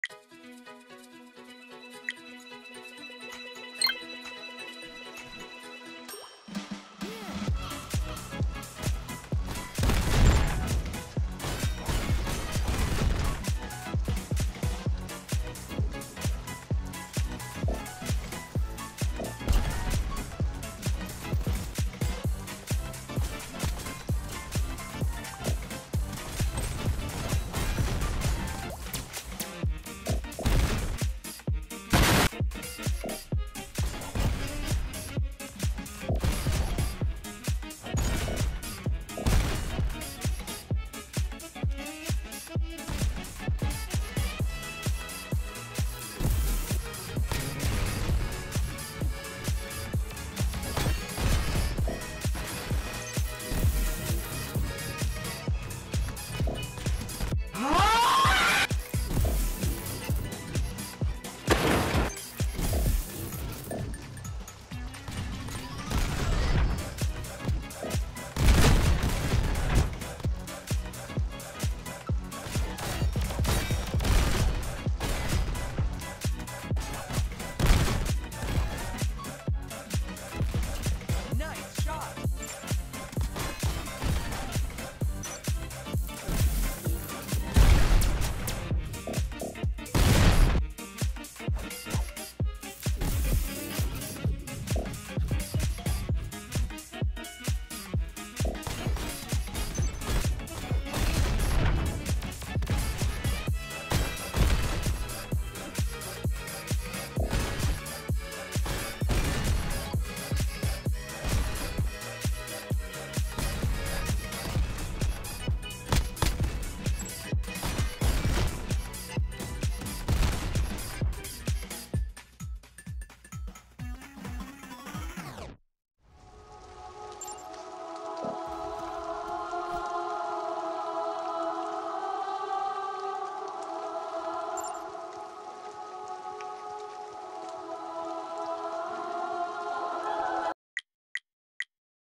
This Is What Happens When Gamedevs Are Bored
Adding a sword might have been the best bad idea I've ever had.

thank you for eating my video with your eyeballs.